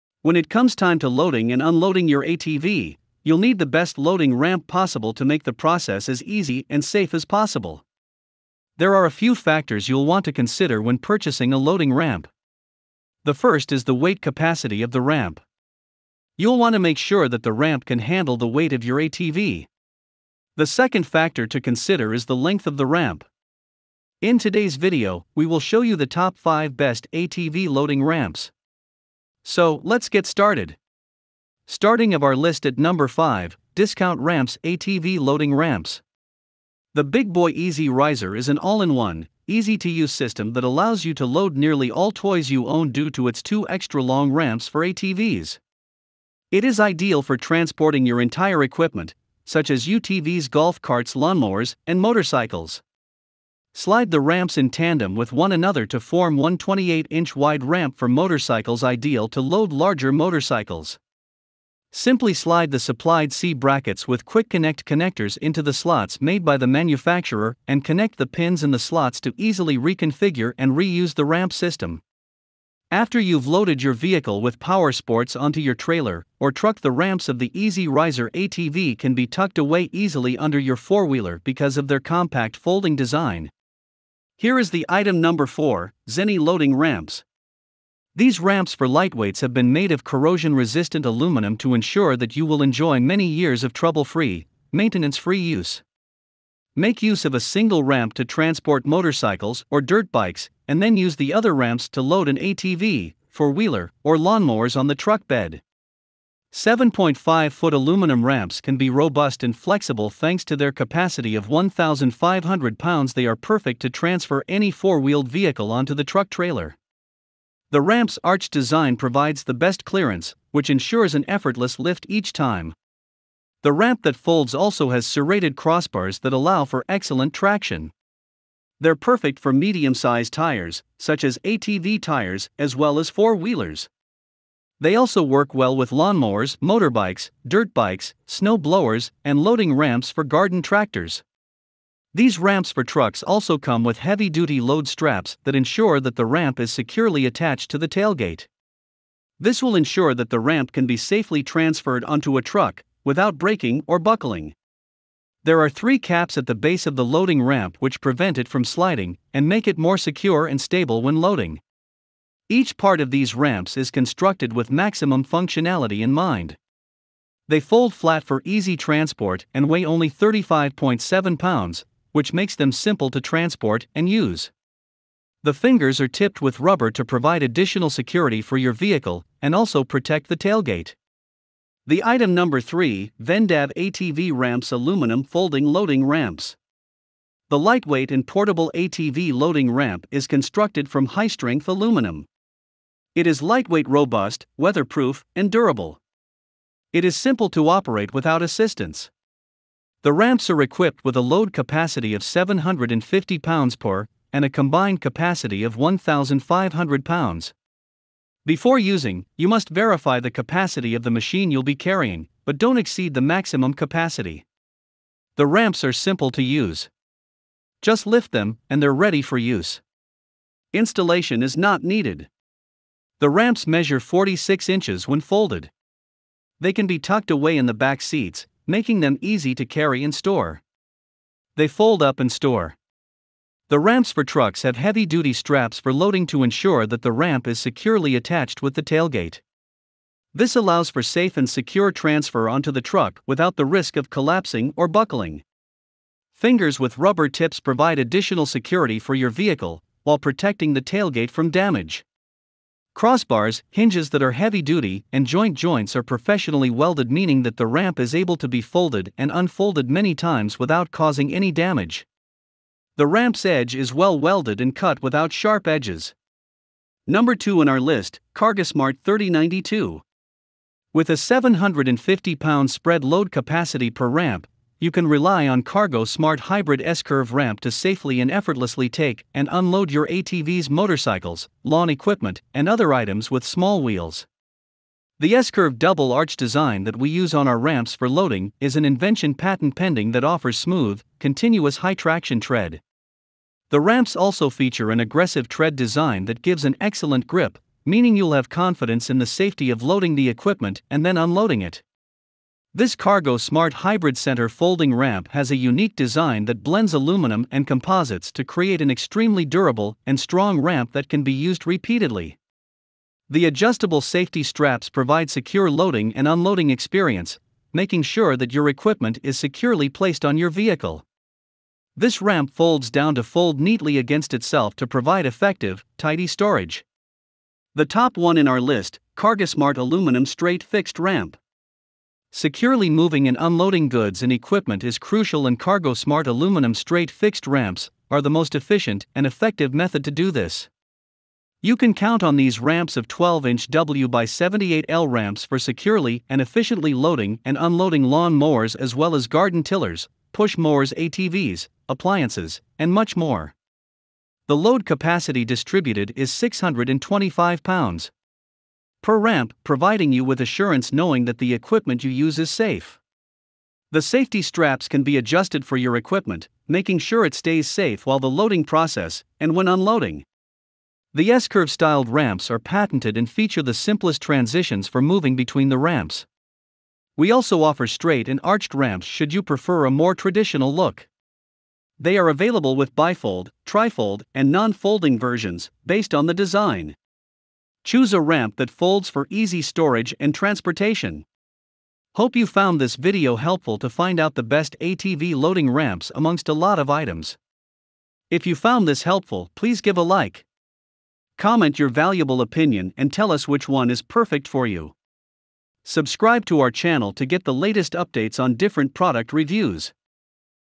Top 5 Best ATV Loading Ramp
5. Discount Ramps ATV Loading Ramps

4. ZENY Loading Ramps

3. VENDAV ATV Ramps Aluminum Folding Loading Ramps

2. CARGOSMART 3092

1. CARGOSMART Aluminum Straight Fixed Ramp